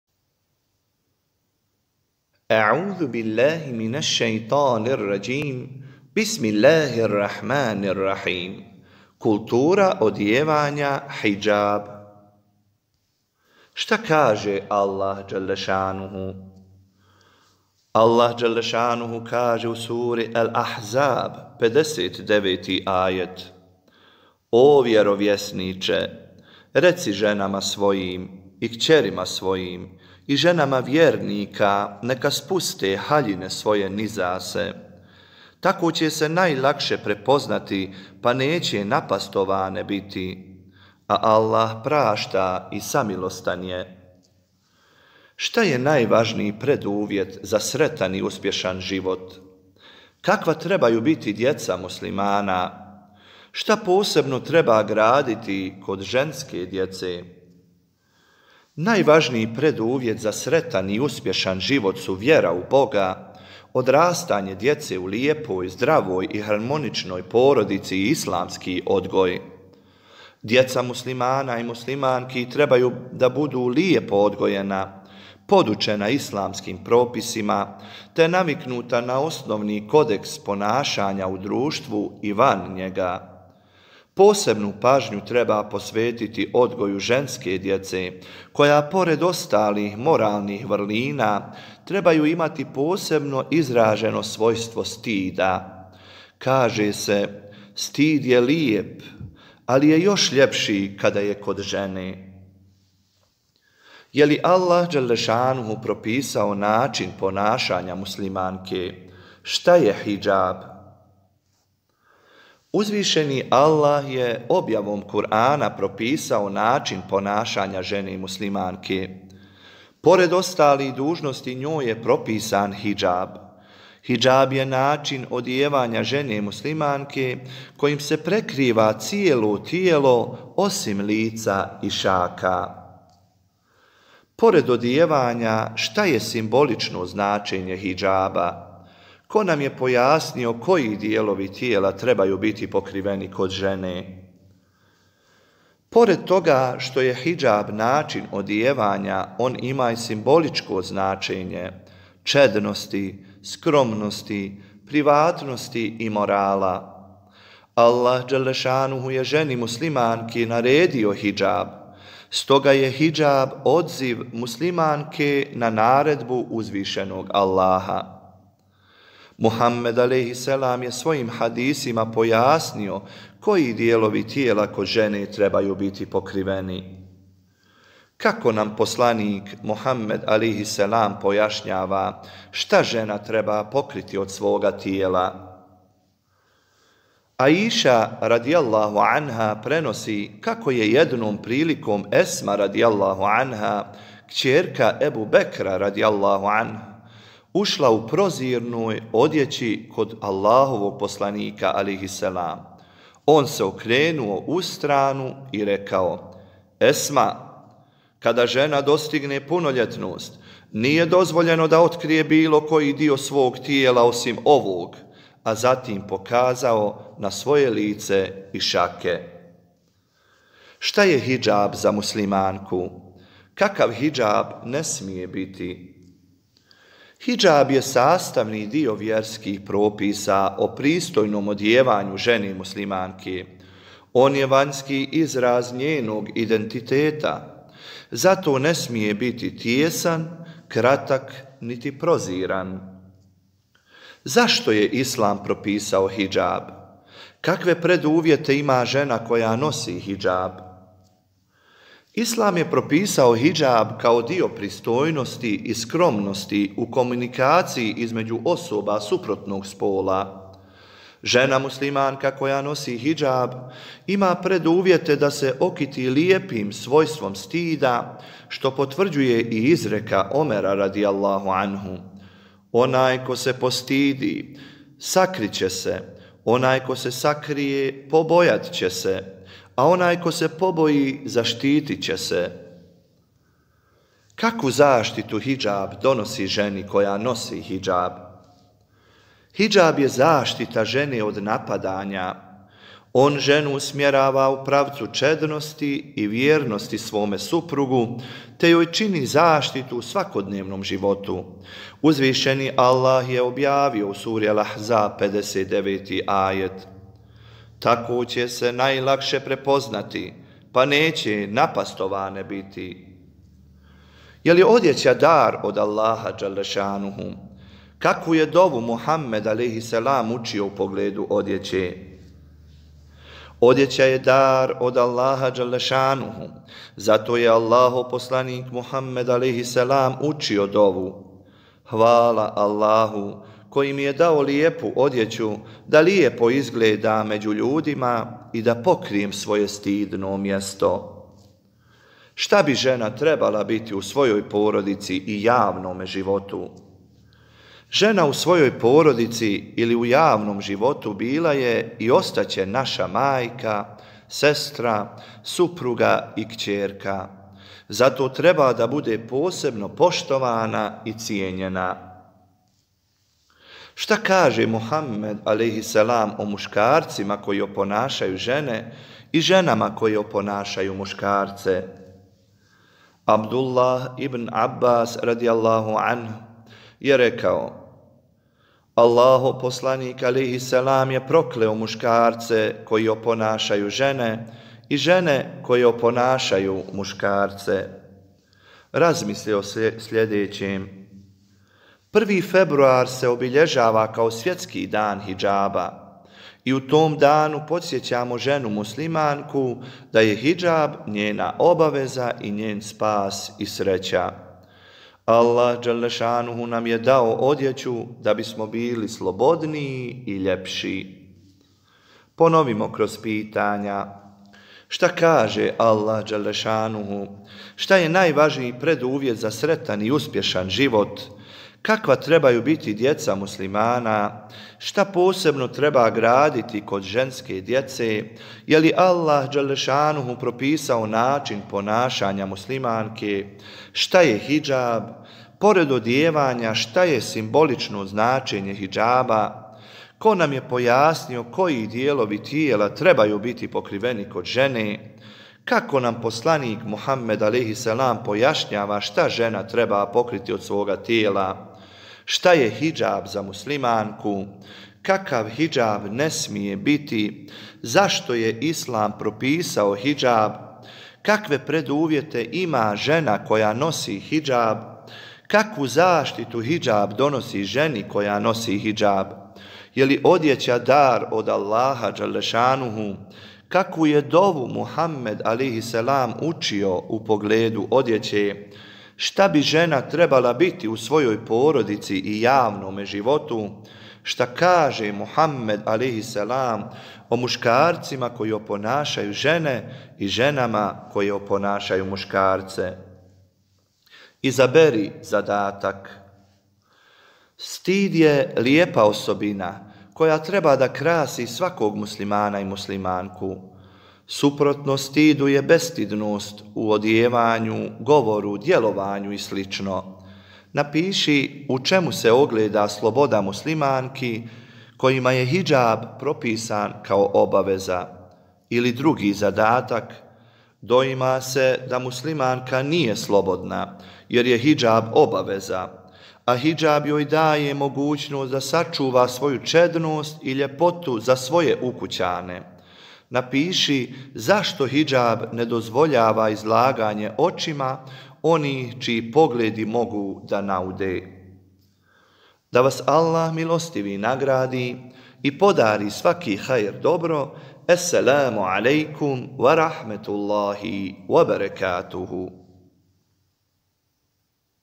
Kultura odijevanja - Hidžab! (Ilmihal 3)
ONLINE MEKTEB! O kulturi odijevanja muslimanke - o hidžabu; treći nivo mektebske pouke; Ilmihal 3! Govori Mensur-ef. Imamović, imam u džematu Tarevo, Medžlis IZ-e Kladanj!